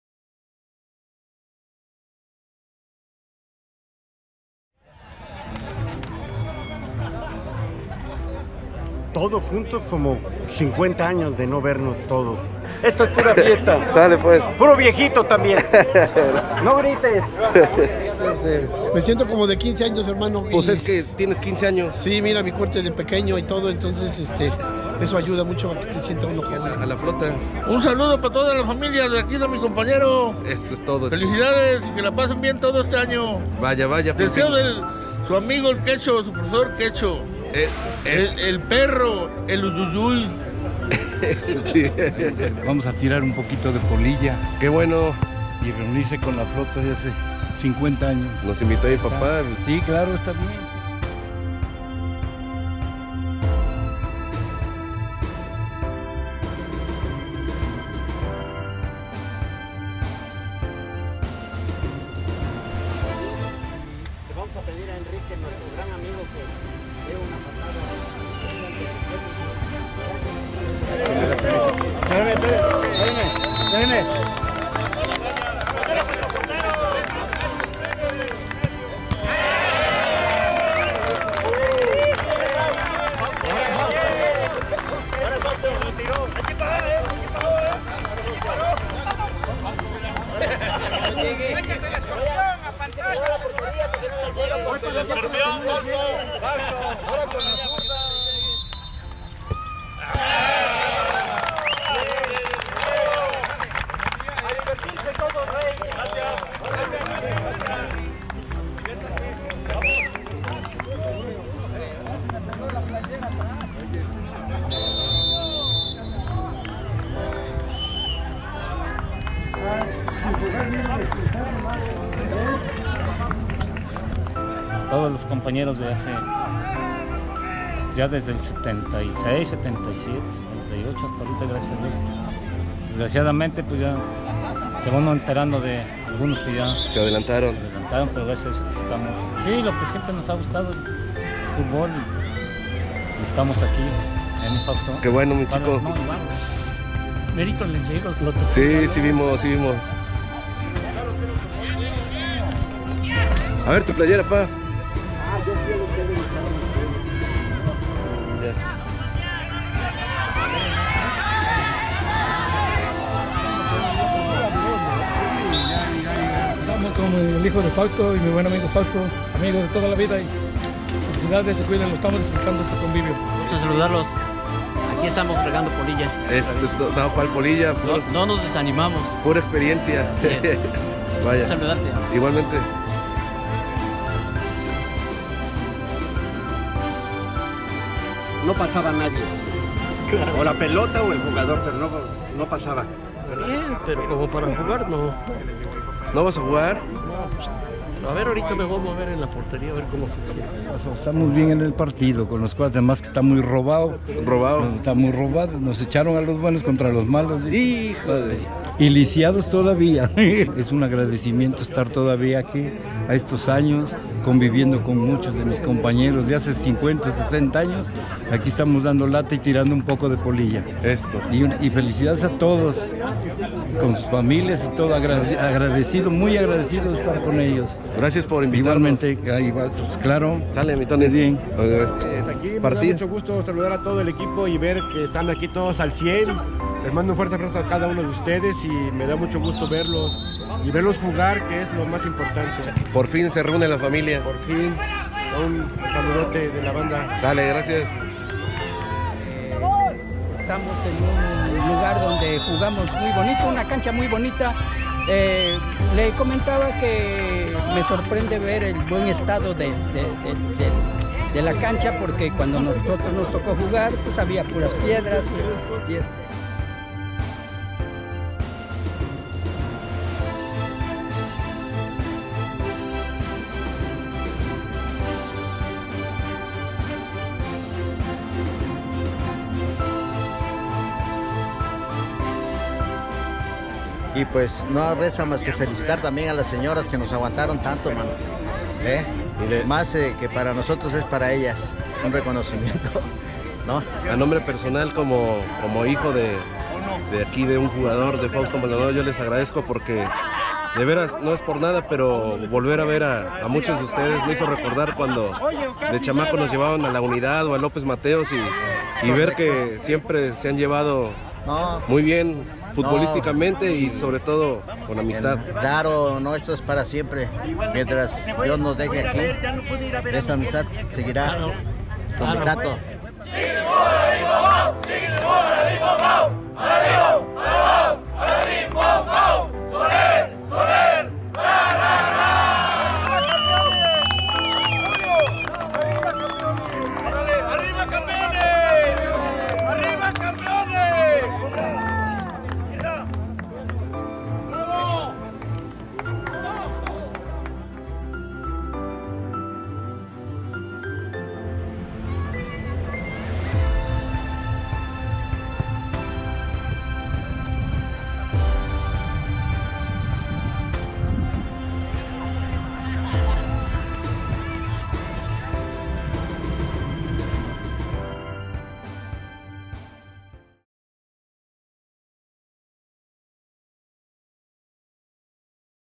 REUNIÓN DEPORTES SOLER
Reunión de amigos que no jugaban desde hace 50 años juntos, mi padre es uno de ellos.